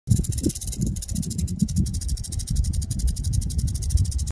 Mohave Green Rattlesnake.
The mohave green has either a venom type a or type b, type b is similar to other rattlesnakes, as in it causes tissue necrosis. etc. type a, is neurotoxin and one of the most potent venom in the world. My phone didn't do a good job in showing the green tint.